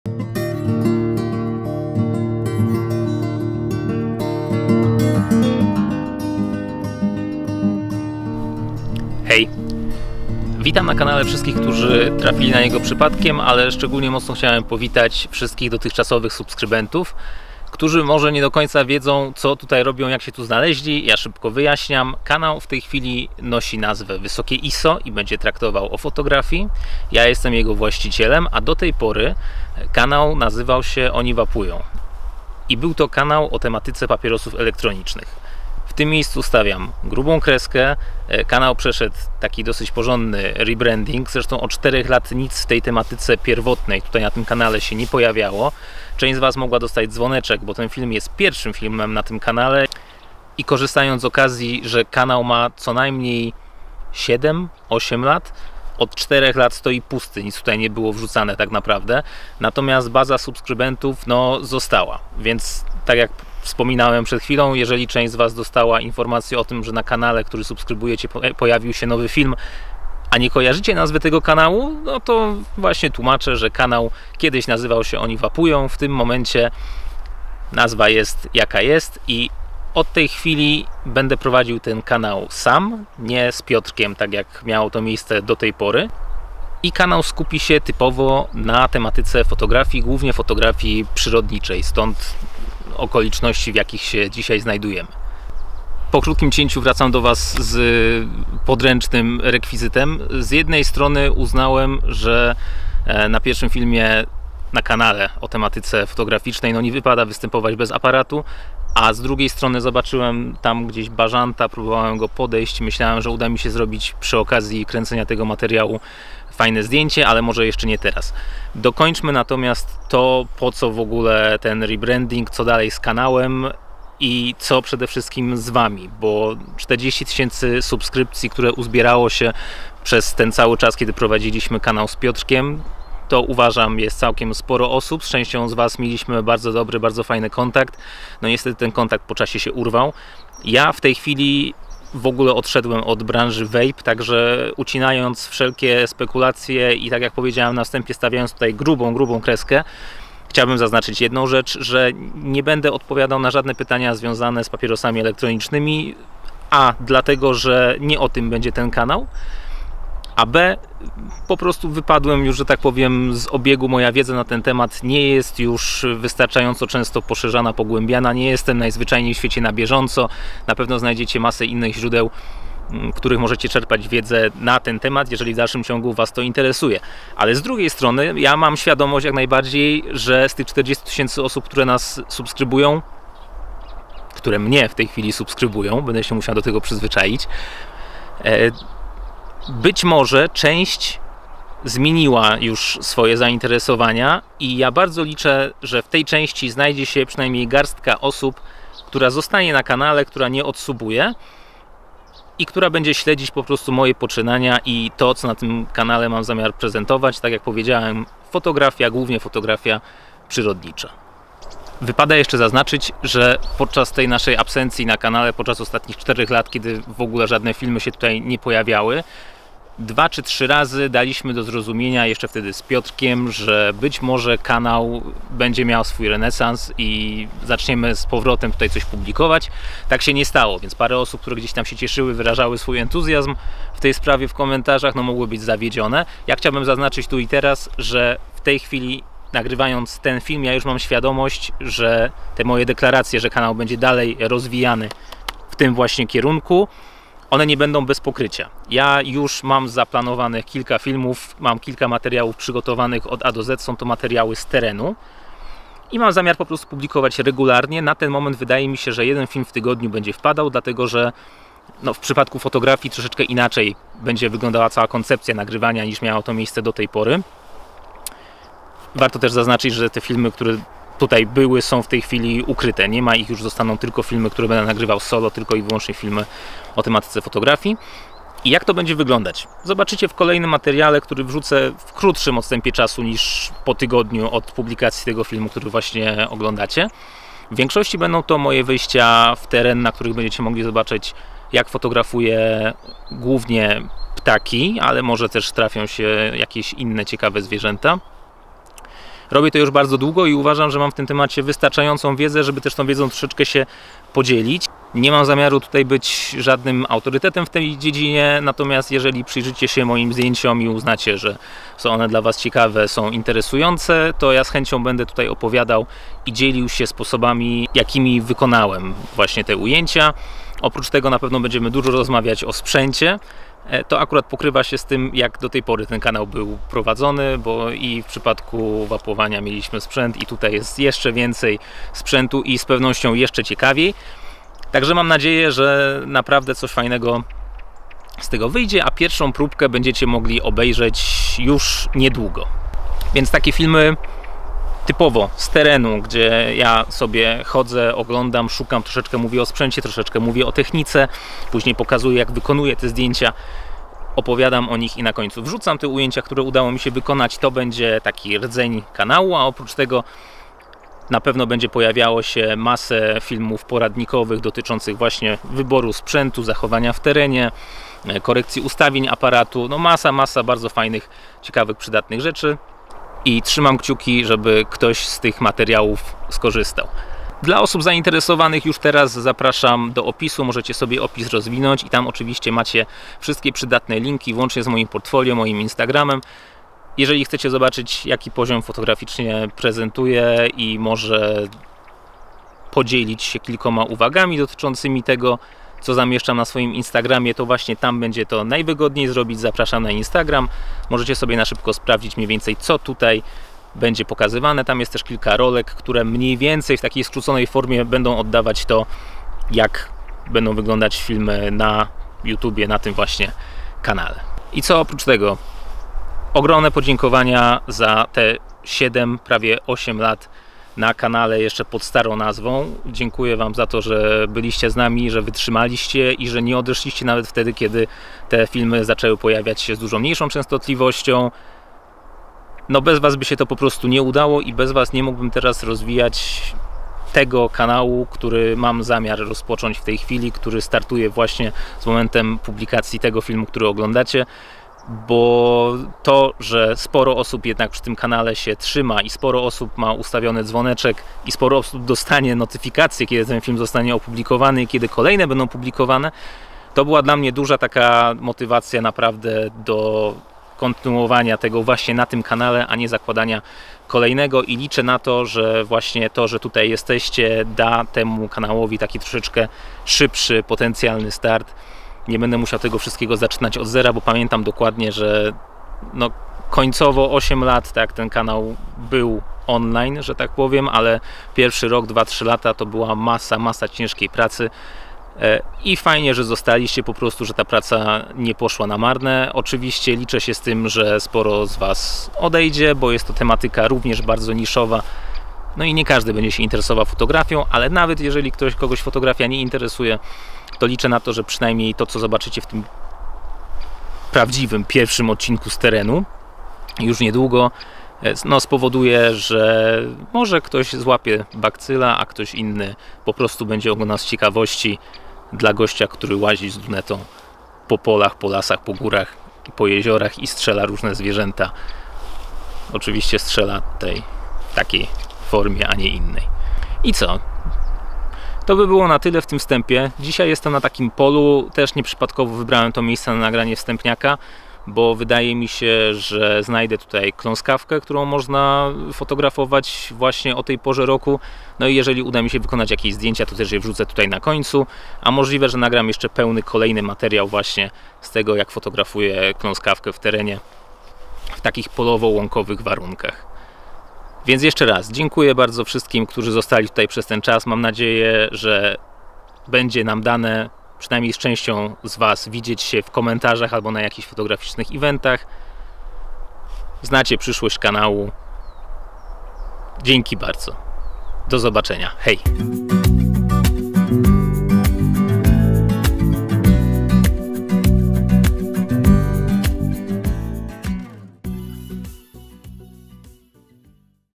Nowy kanał o fotografii - Fotografia przyrodnicza
Hej! Witajcie na nowym/starym kanale! Prowadzący bez zmian, a jedynie tematyka nieco inna! Sporo osób oglądało nas jeszcze gdy kanał nosił nazwę "Oni Wapują". W tym debiutanckim materiale wyjaśniam co tu się będzie działo dalej... ;)